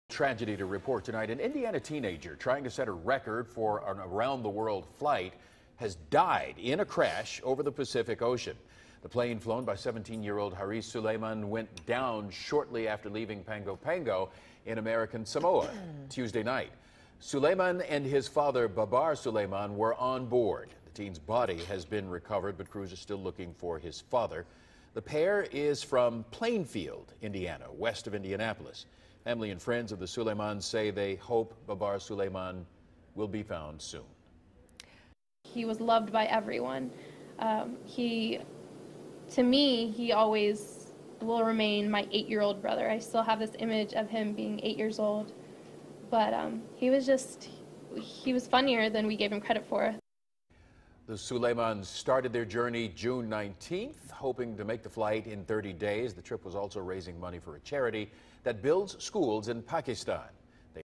Indiana teen dies in place crash over Pacific
Trying to make world record flight with his father